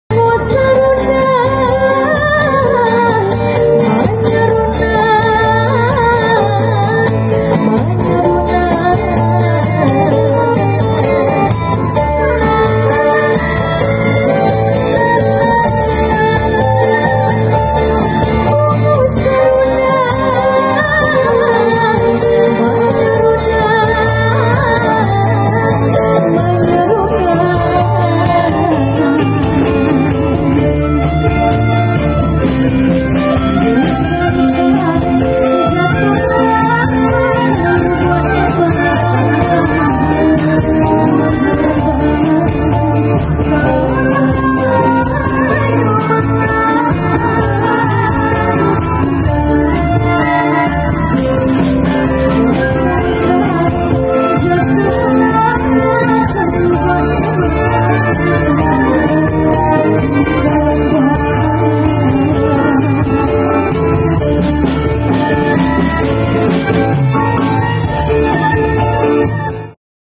Pembukaan Pekan Kebudayaan Daerah (PKD) Sumatera Barat 2024
Pembukaan Pekan Kebudayaan Daerah (PKD) Sumatera Barat 2024 hari Pertama

by : @tamanbudayasumbar